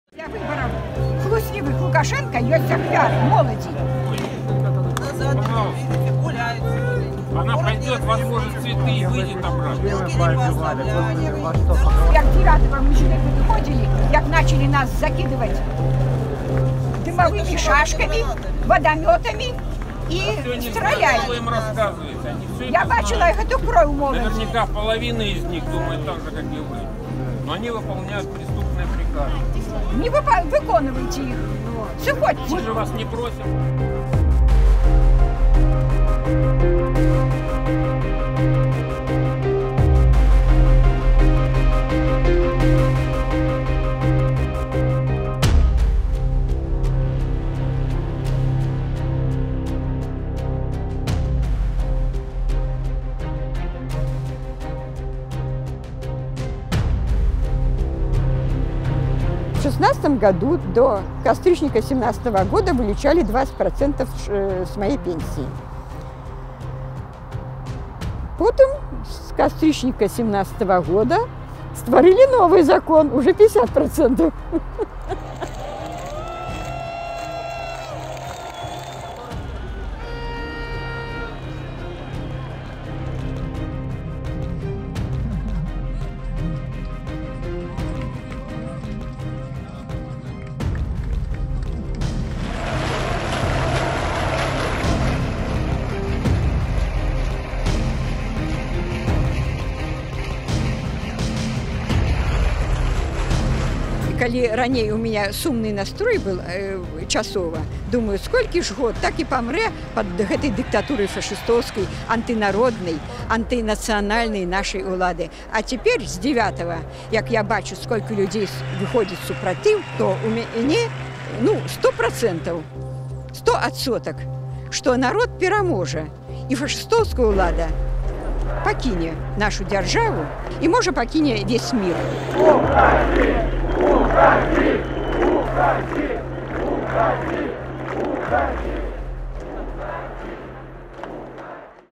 Meet The 73-Year-Old Great-Grandmother Defying Lukashenka In Belarus
Nina Bahinskaya cuts a frail but resolute figure amid the crowds protesting the presidential election results in Belarus that are widely seen as rigged. She's been a regular feature at various demonstrations since 1988.